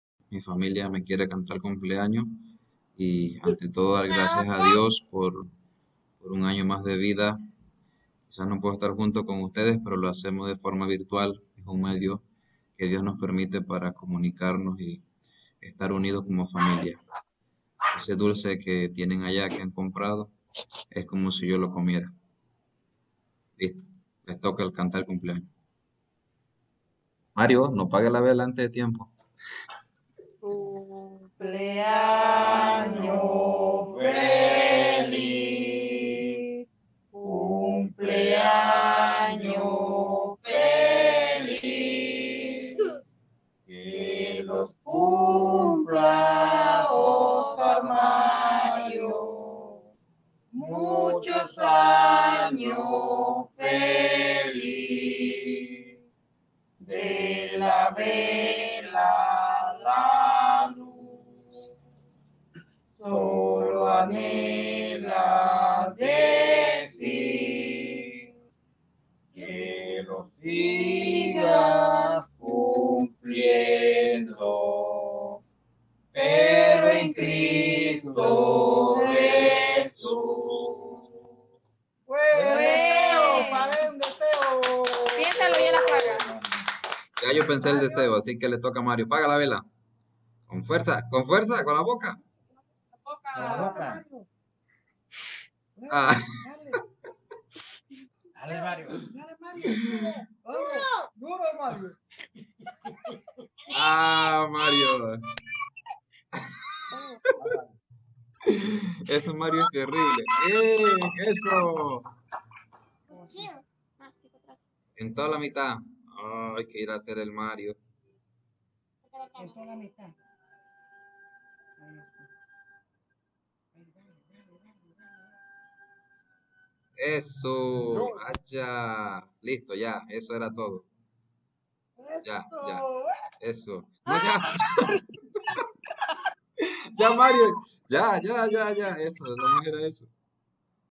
🎂 Mi Cumpleaños #...🤣🤣
Agradezco a Dios por permitirme cumplir un año más de Vida.  Al igual que a todos ustedes , que me han mostrado su cercanía y acompañamiento a través de la Oración.  Mi Señor me les siga bendiciendo y fortaleciéndoles  en la Fe.  Gracias querida Familia por ese detalle.
Si es primera vez que visitas mi canal. De esta formas contribuyes a que poco a poco vallamos creciendo.